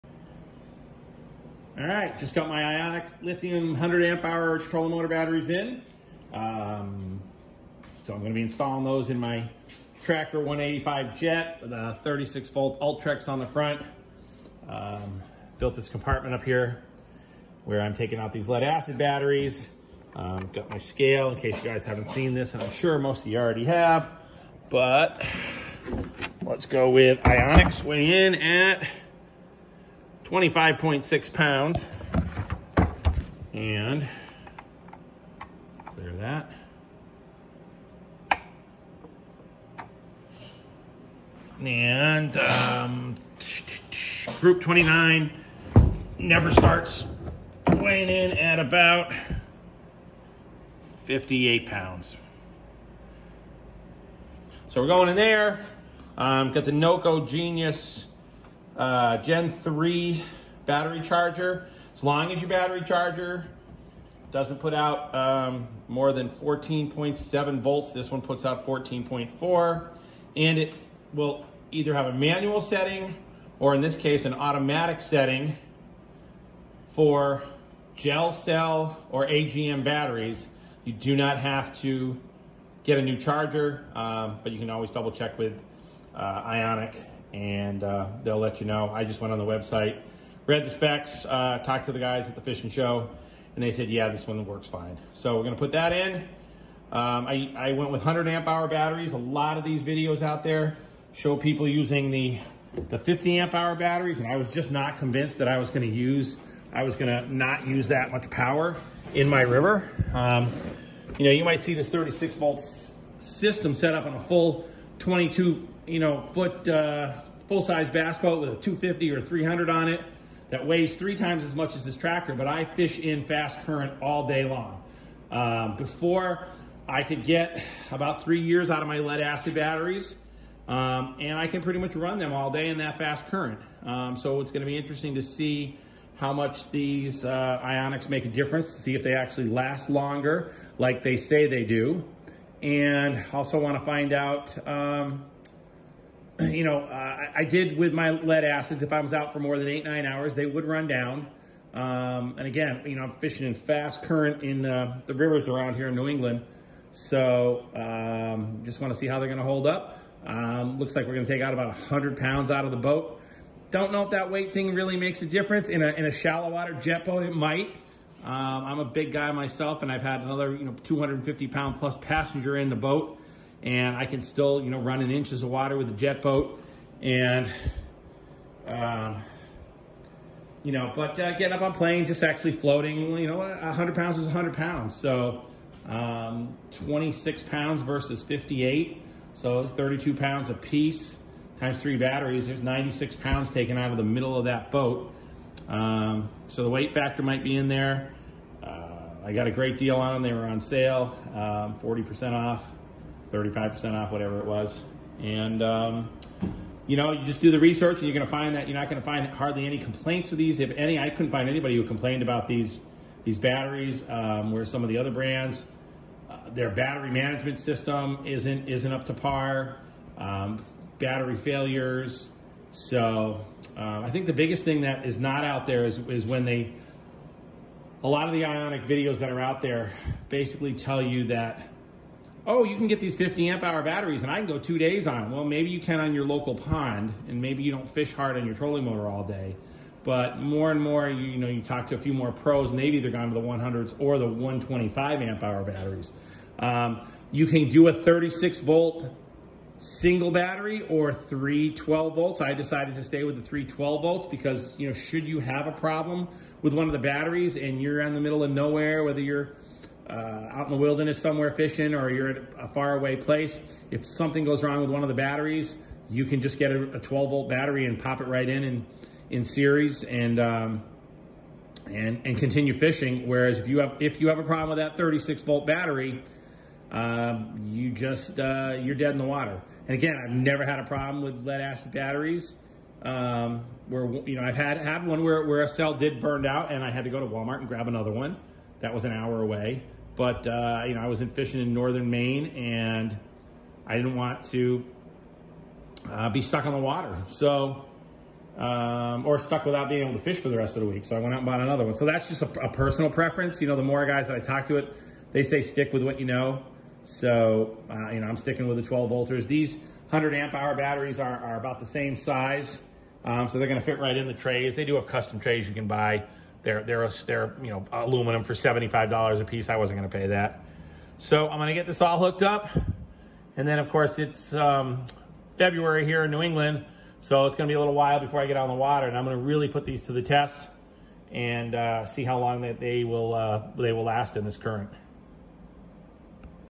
Ionic lithium 100ah battery install part 1 of 3
I’m not sponsored and I watched every video I could find on lithiums before I decided to go with Ionics. Love them so far!